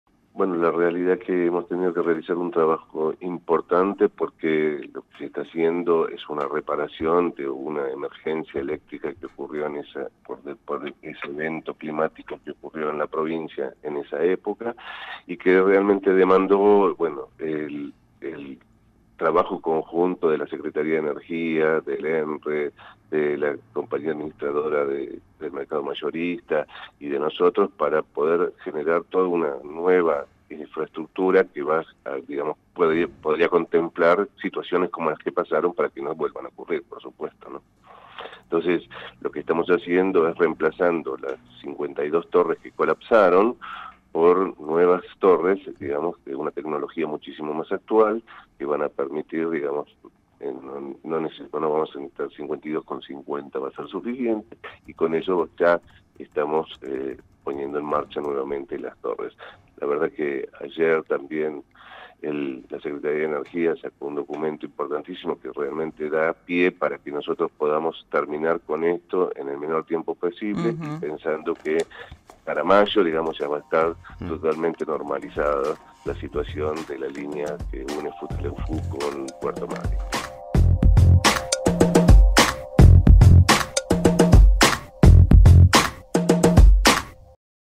Restaurarán la línea eléctrica desde Futaleufú a Madryn
Se trata de los trabajos que restaurarán operativamente la línea de transmisión que va desde Futaleufú hasta Puerto Madryn. En dialogo con Cadena Tiempo Fernando Guerra, gerente de Transpa, dijo que "vamos a reemplazar las 52 torres por nuevas torres de alta tecnología", agregando que "para mayo la línea estará lista".
📢 Fernando Guerra - Gerente Transpa